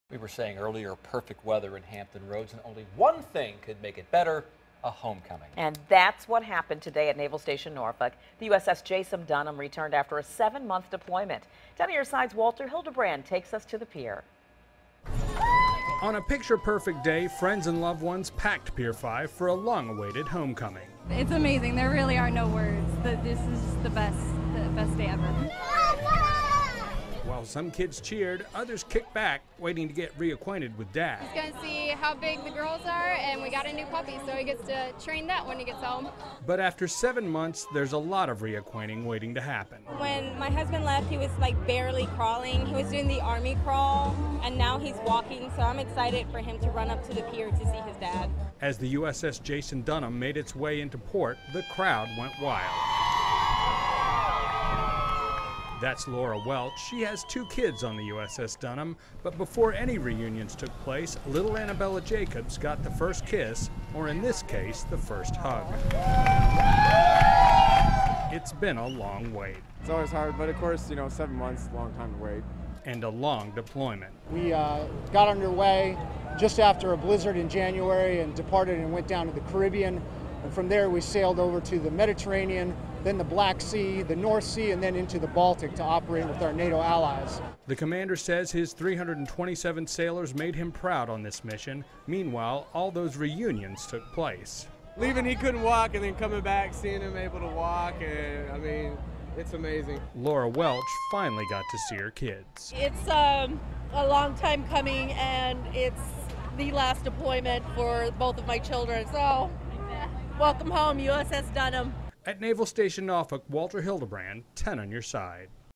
USS Jason Dunham returns to NSN
The crew of the USS Jason Dunham returned to blue skies and a very excited crowd Friday morning.